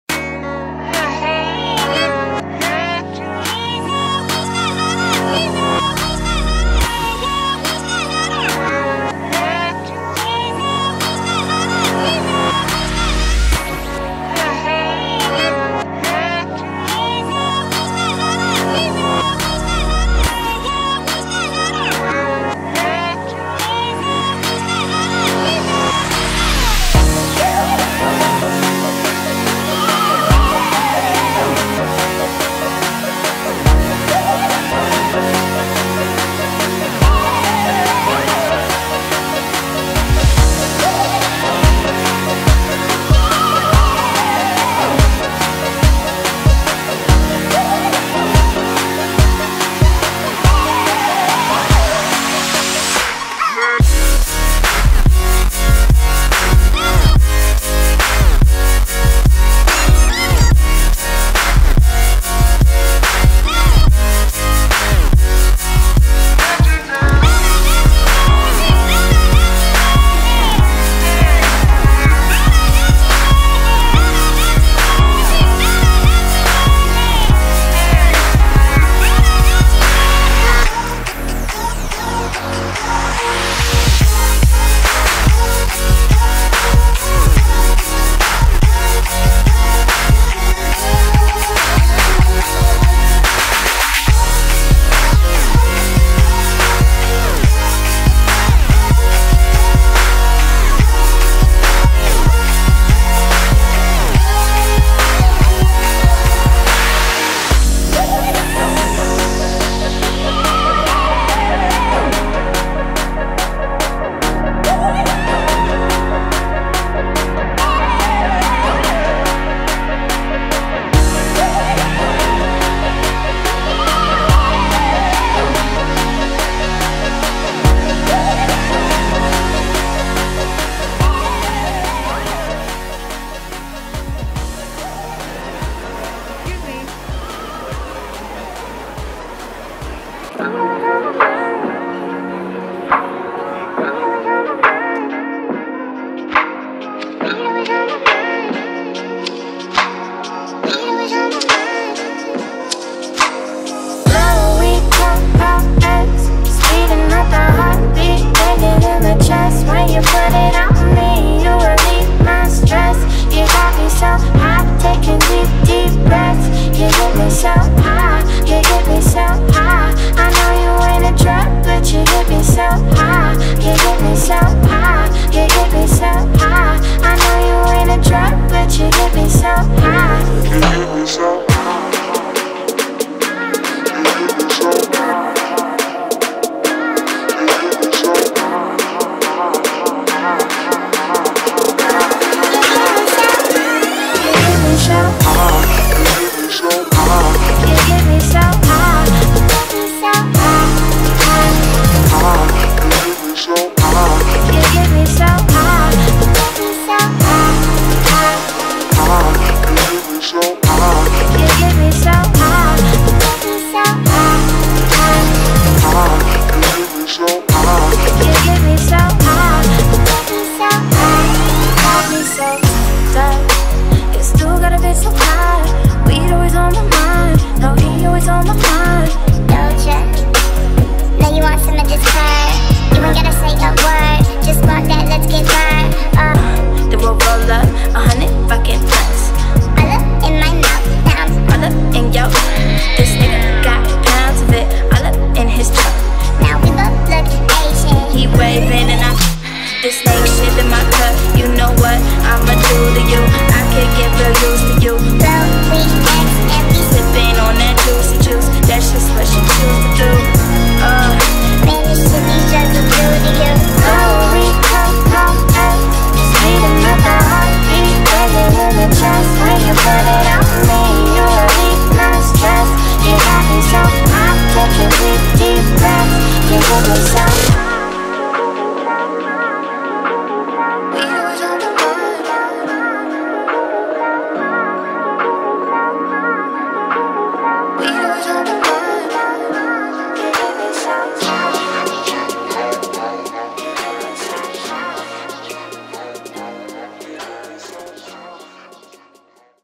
Anime Expo 2014
This is my first year doing a vlog for AX! I was lucky enough to see Amie Lynn and Tasha & Doremi from Spiral Cats, all the other amazing cosplayers, old friends, and new! All in all, it was another great year. Hope to see you again next time!